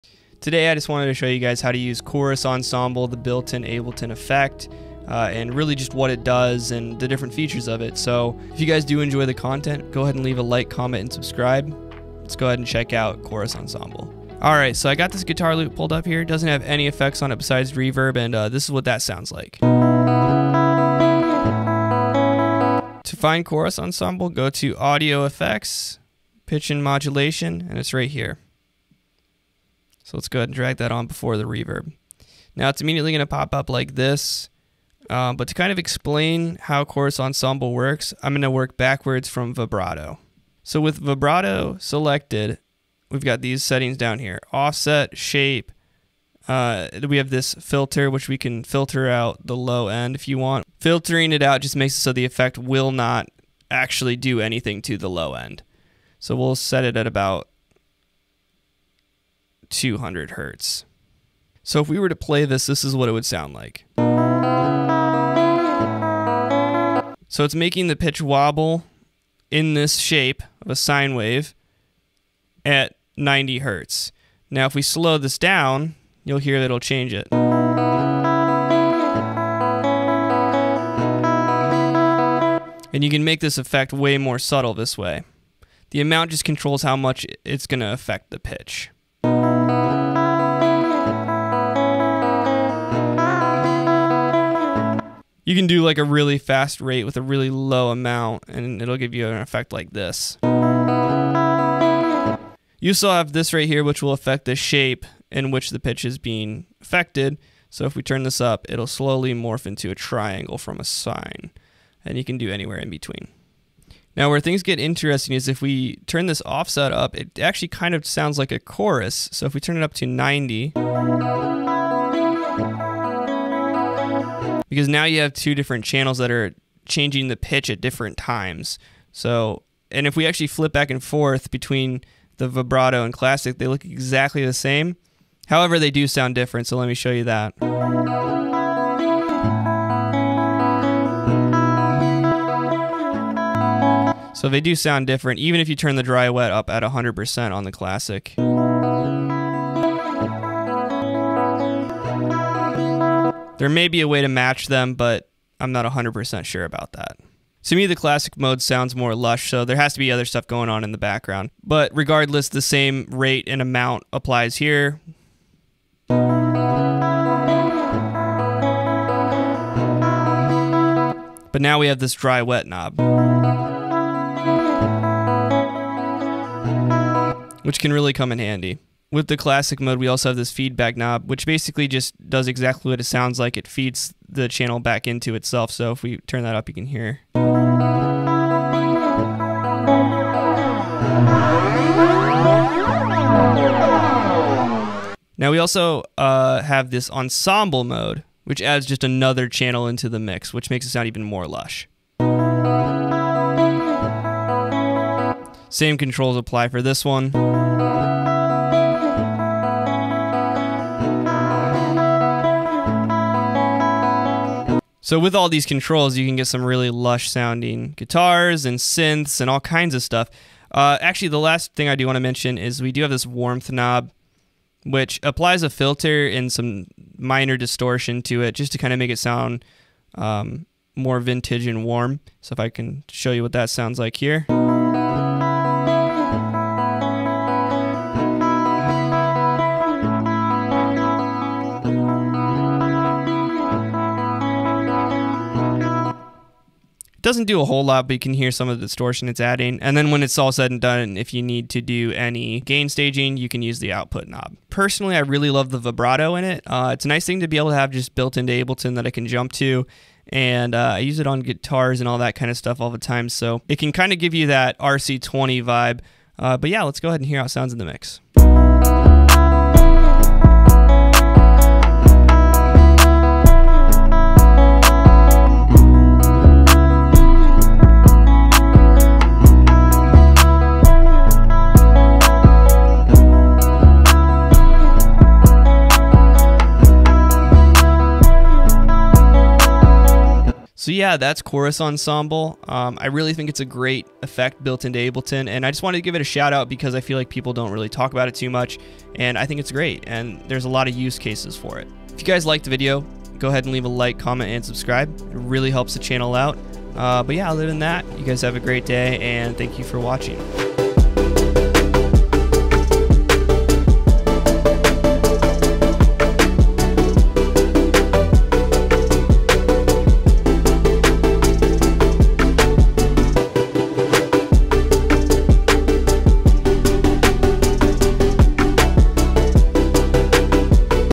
How to use Chorus Ensemble in Ableton 11
Chorus Ensemble is more than enough to fill most chorus and tremolo needs and it comes with Ableton for free. If you have not checked it out or used it then you should definitely give it a try. I hope this tutorial answered all the questions you may have about Chorus Ensemble!

Ableton Push
Apollo Twin X Duo
Prophet Rev2
My Camera
My Lens
My Mic
My Studio Monitors
My Headphones
My Thunderbolt Dock

God Bless 🙏